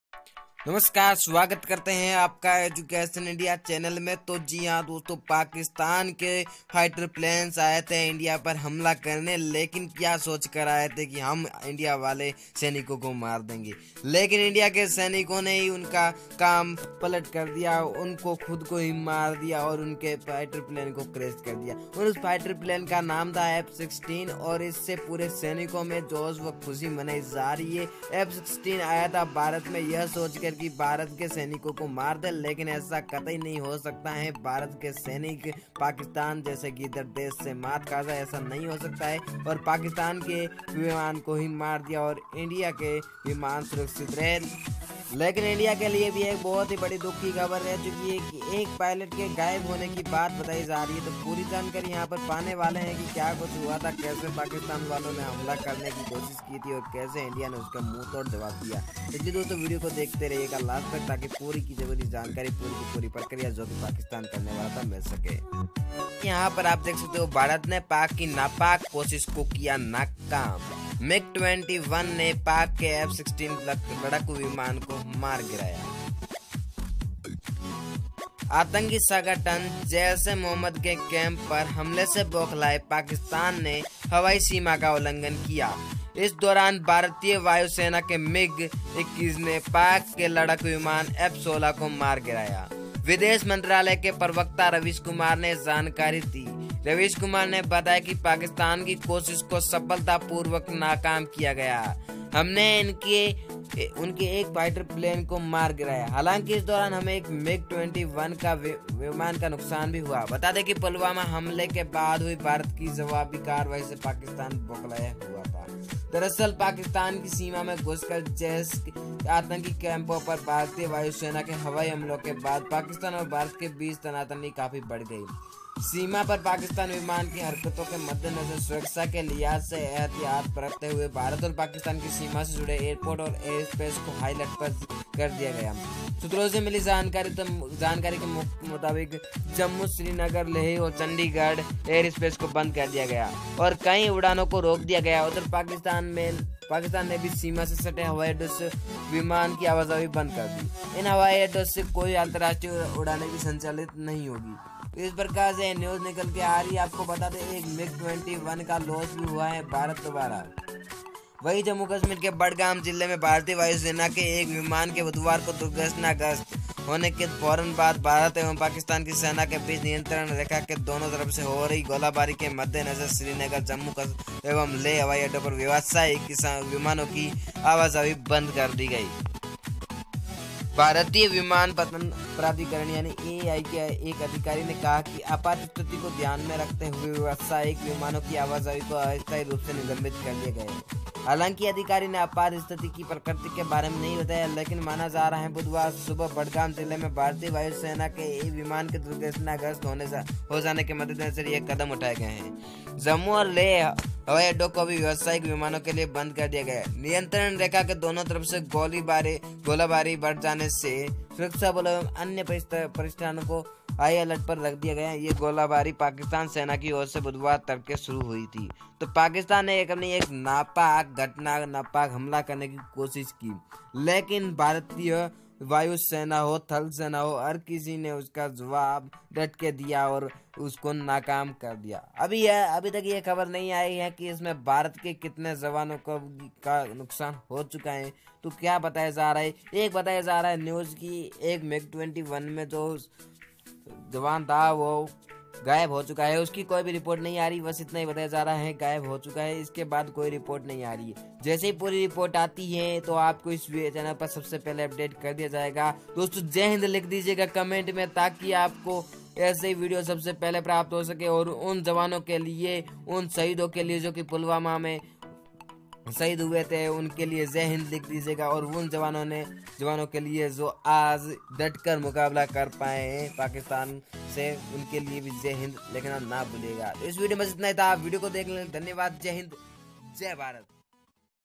भारत ने मार गिराया पाकिस्तानियों को// आये थे हमारे जवानों को मारने खुद मारे गए
नई दिल्ली: आतंकी संगठन जैश-ए-मोहम्मद के कैंप पर हमले से बौखलाए पाकिस्तान ने हवाई सीमा का उल्लंघन किया. इस दौरान भारतीय वायुसेना के मिग-21 ने पाक के लड़ाकू विमान F-16 को मार गिराया. विदेश मंत्रालय के प्रवक्ता रवीश कुमार ने यह जानकारी दी. रवीश कुमार ने बताया कि पाकिस्तान की कोशिश को सफलतापूर्वक नाकाम किया गया और हमने उनके एक फाइटर विमान को मार गिराया. हालांकि, इस दौरान हमें एक मिग-21  विमान का नुकसान हुआ. बता दें कि पुलवामा हमले के बाद हुई भारत की जवाबी कार्रवाई से पाकिस्तान बौखलाया हुआ है.

दरअसल, पाकिस्तान की सीमा में घुसकर जैश के आतंकी कैंपों पर भारतीय वायुसेना के हवाई हमले के बाद पाकिस्तान और भारत के बीच तनातनी काफी बढ़ गई है. सीमा पर पाकिस्तानी विमान की हरकतों के मद्देनजर सुरक्षा के लिहाज से एहतियात बरतते हुए भारत और पाकिस्तान की सीमा से जुड़े एयरपोर्ट और एयरस्पेस को हाई अलर्ट पर कर दिया गया है. सूत्रों से मिली जानकारी के मुताबिक जम्मू, श्रीनगर, लेह और चंडीगढ़ एयर स्पेस को बंद कर दिया गया है और कई उड़ानों को रोक दिया गया है. उधर पाकिस्‍तान ने भी सीमा से सटे हवाई अड्डों से विमान की आवाजाही बंद कर दी है. इन हवाई अड्डों से कोई अंतरराष्‍ट्रीय उड़ानें भी संचालित नहीं होंगी.